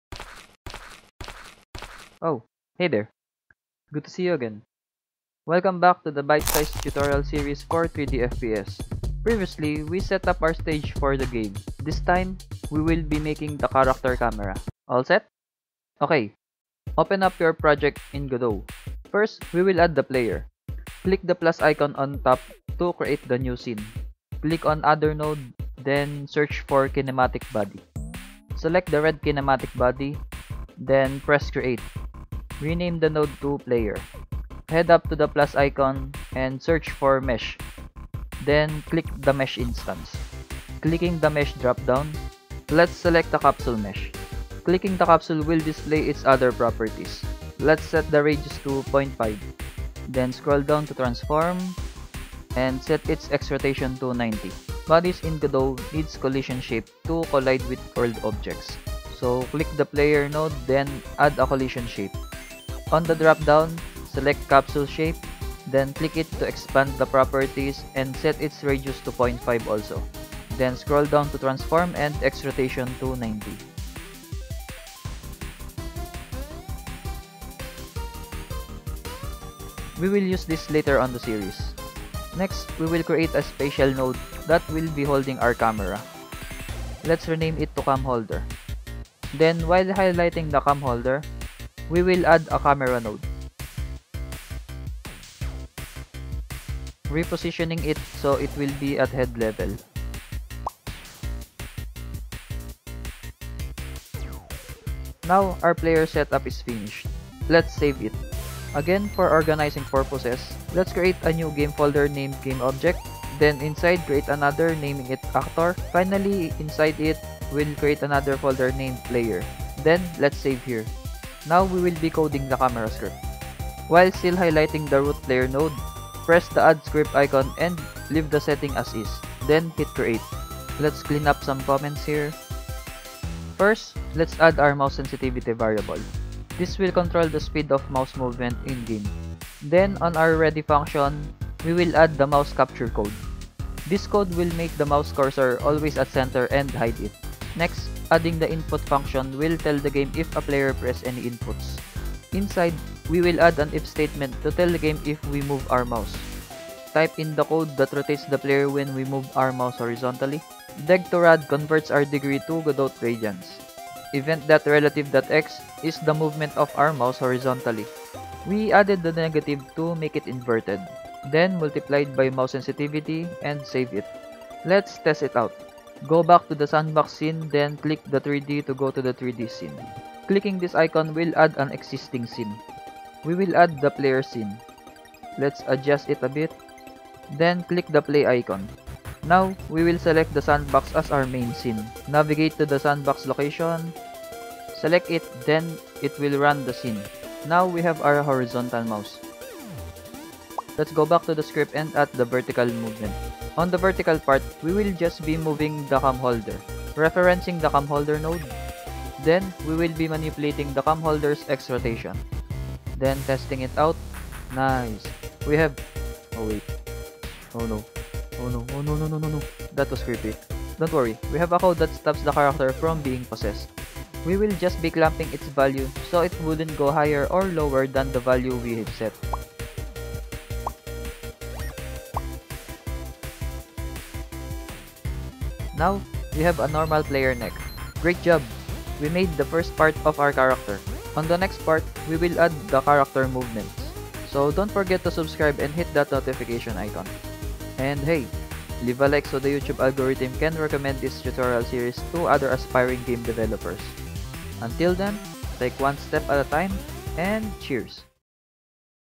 Bite Size Tutorial Basic FPS Pt.2: Player Camera | Godot 3.3
Adding a simple player camera to a fps game